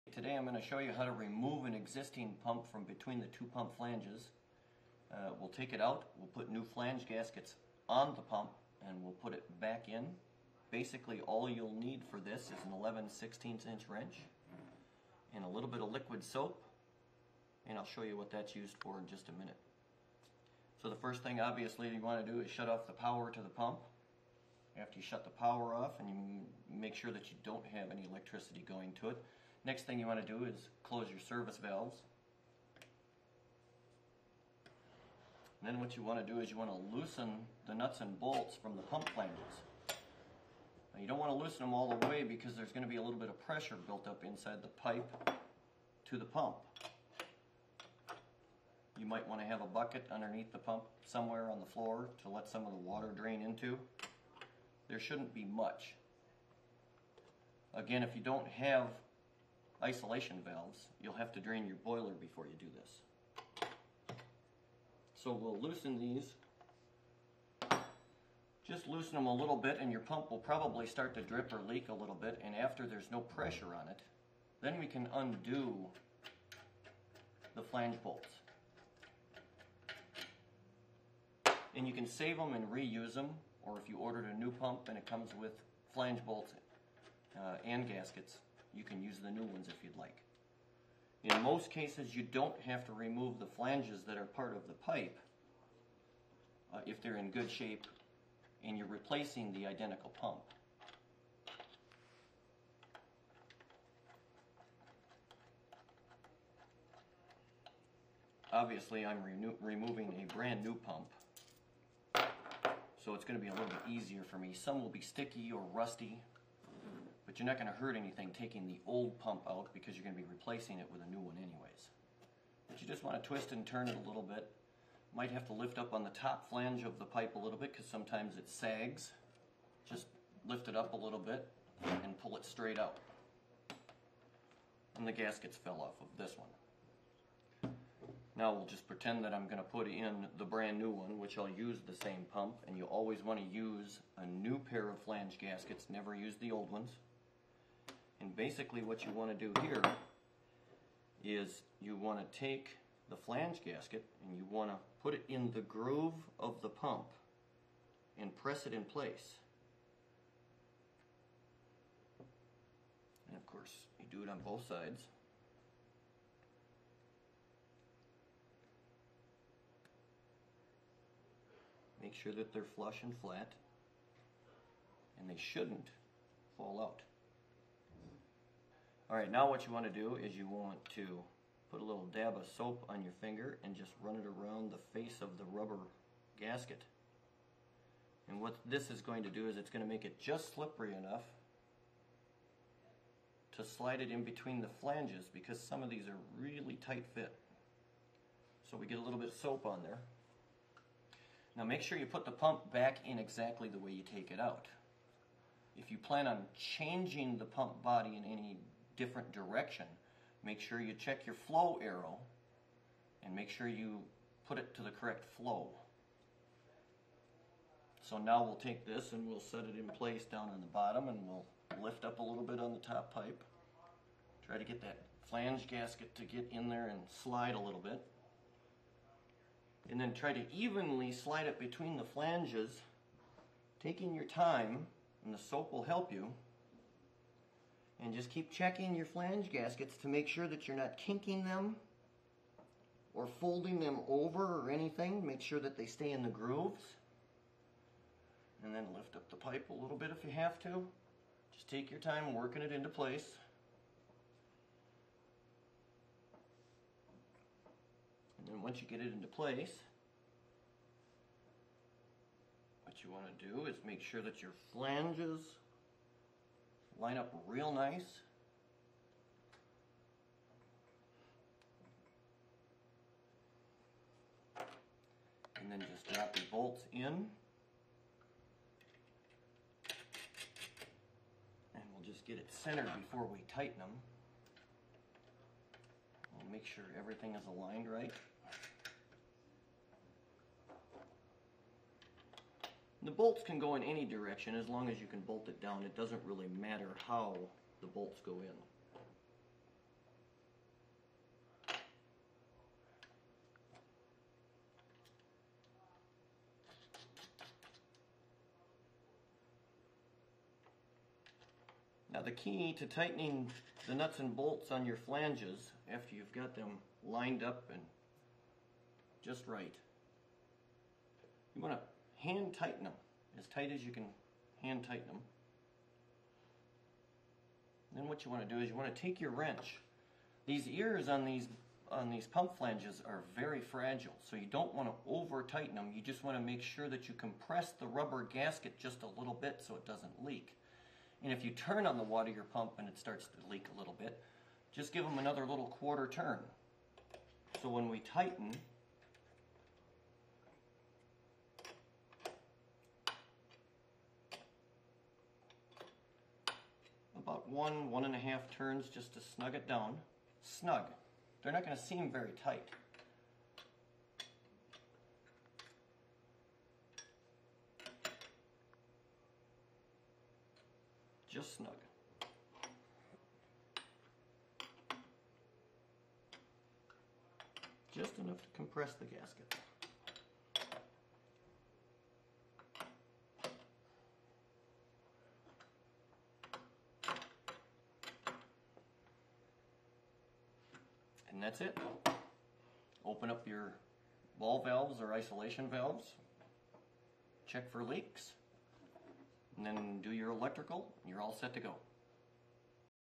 Bell and Gossett NRF-22
How to install a Bell and Gossett pump, series NRF-22.